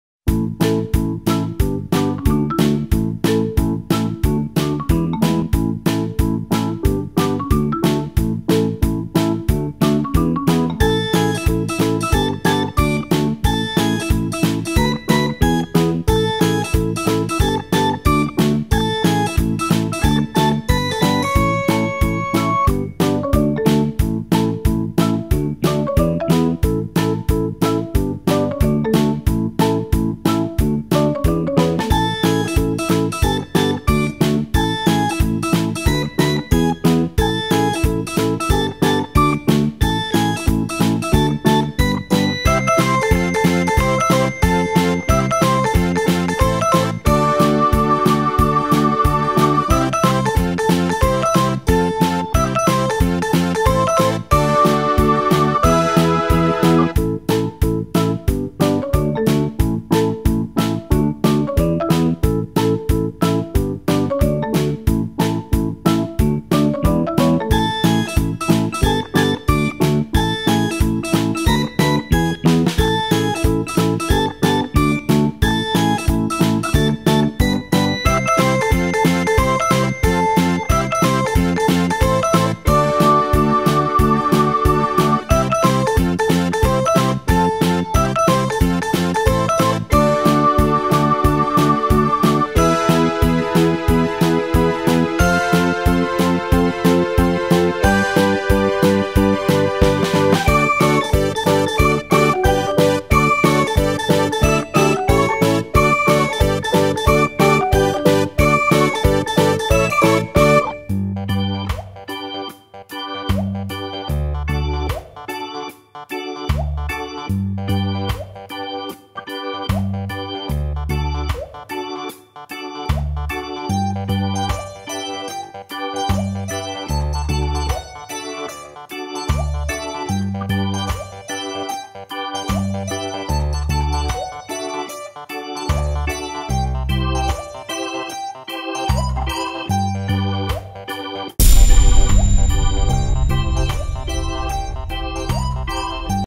dibuja y colorea escoba de bruja y sombrero de bruja para niños - cat color 🧙‍♀
hola amigos
hoy enseño a dibujar una escoba  y sombrero de bruja paso a paso